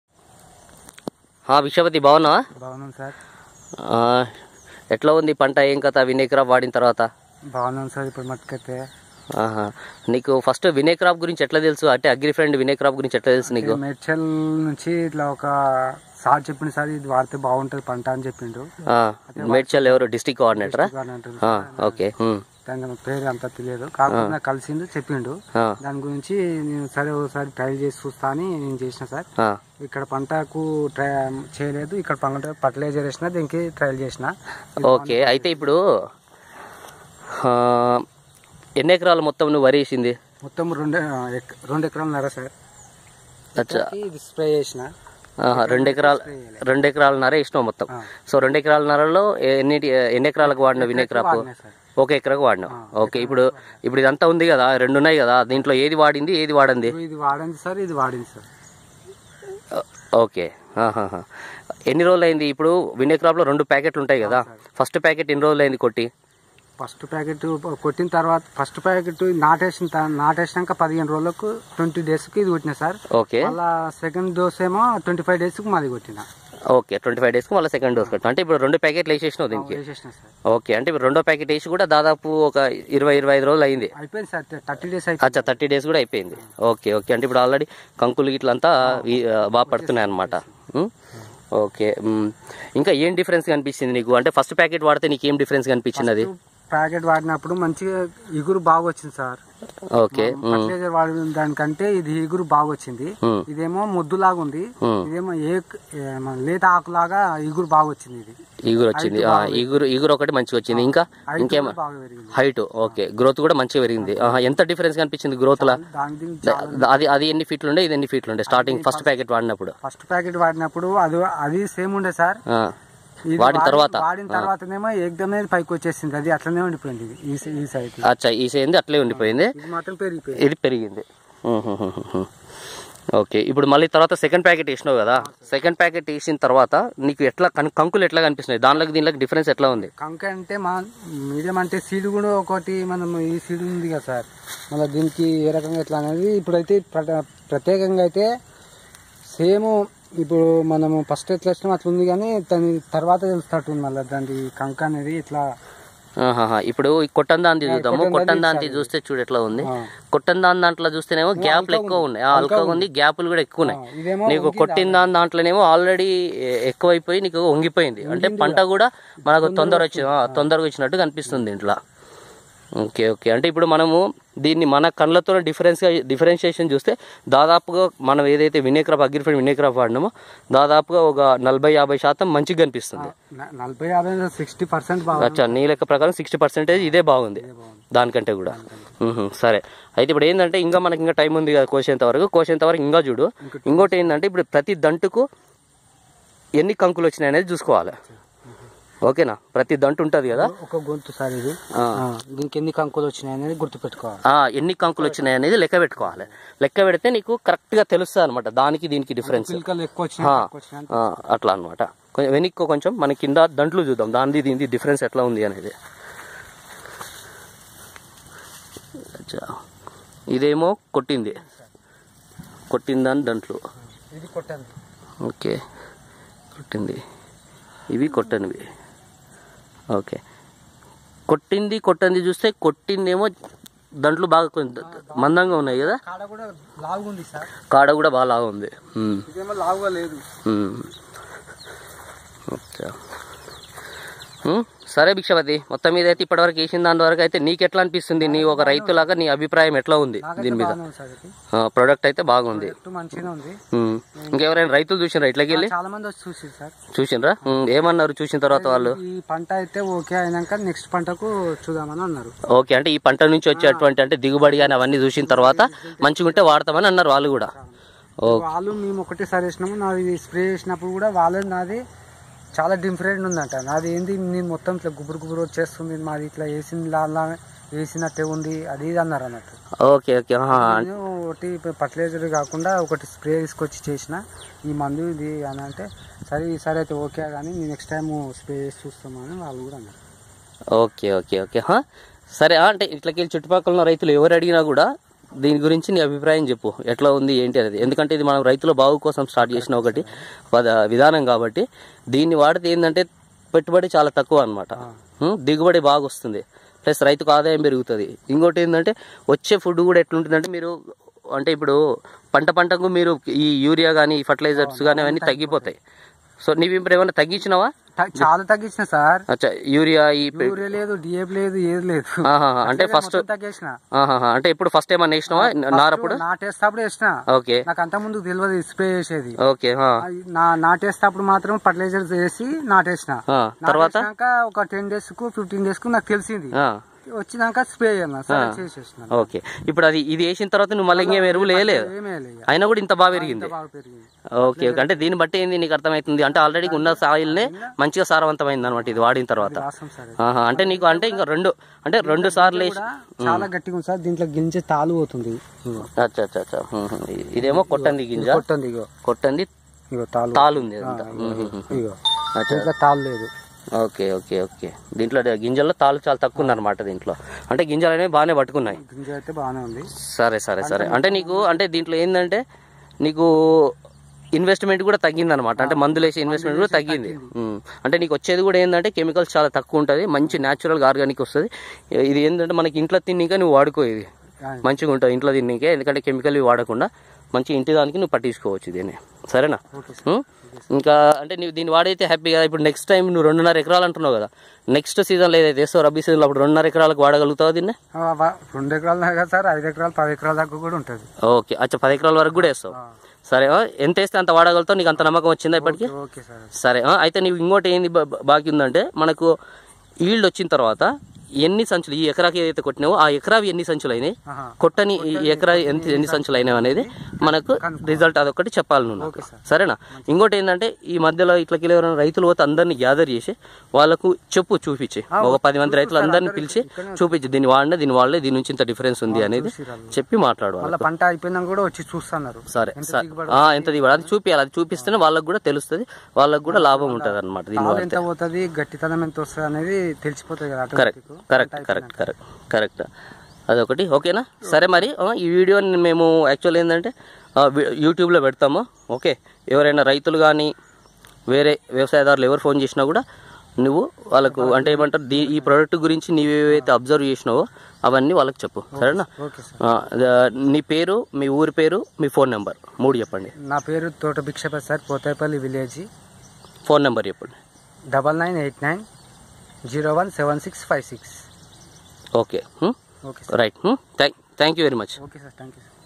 Agrifriend Team Medchal dist mc paddy crop
Paddy crop super results with technology of Agrifriend Win-ea-crop at Medchal District in Telangana State